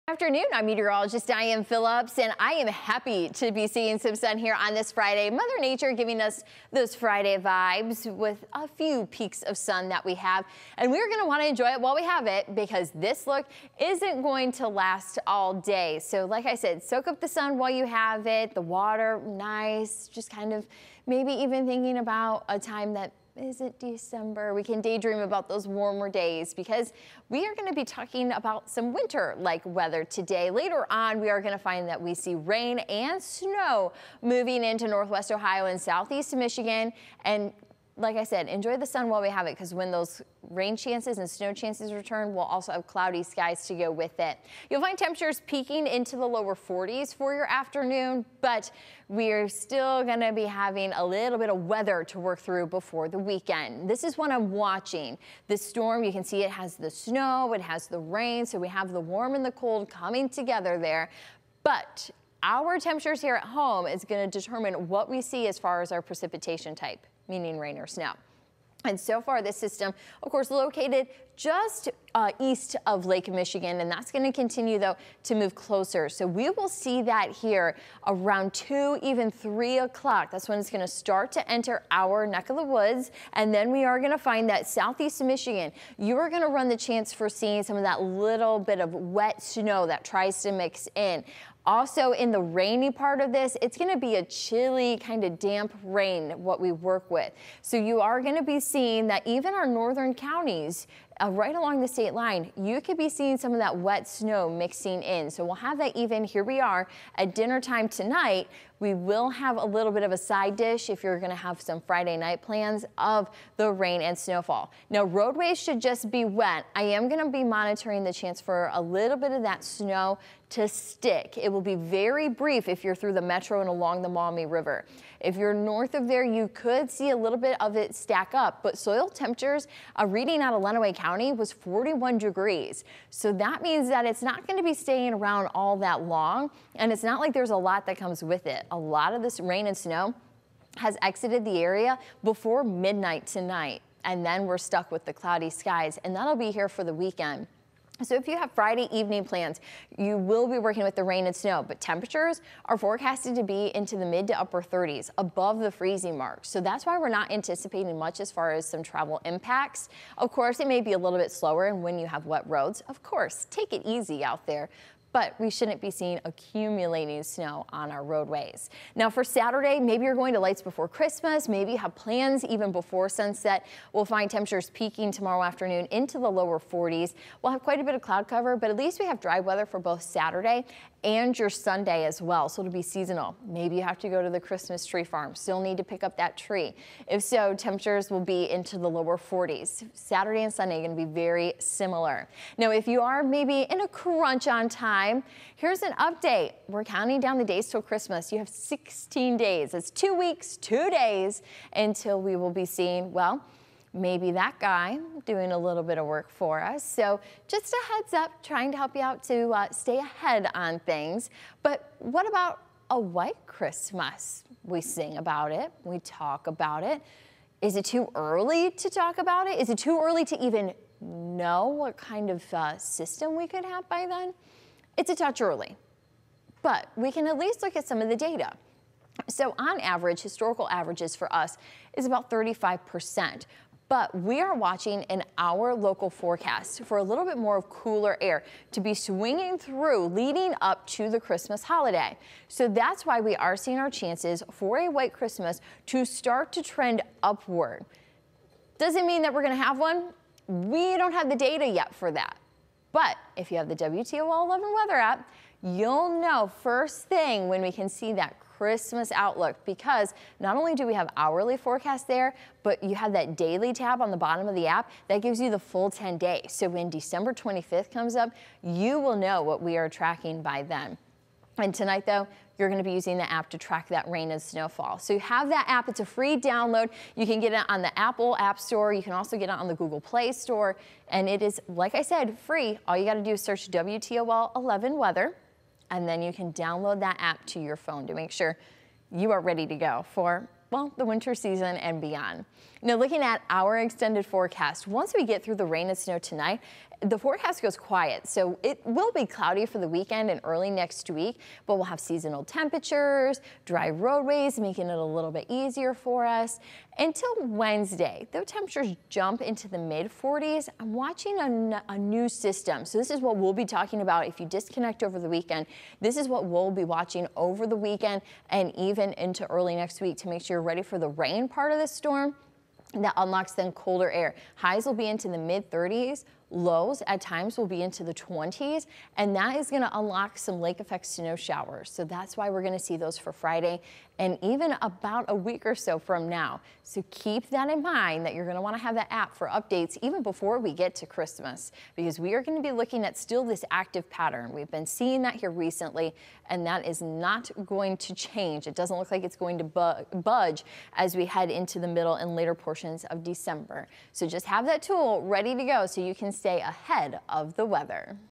What's the weather today? WTOL 11 forecast calls for rain and snow for Friday, Dec. 9
Early sunshine gives way Friday to cold rain and possibly a snowy mix. Mostly dry conditions return for the weekend. Next week turns much colder.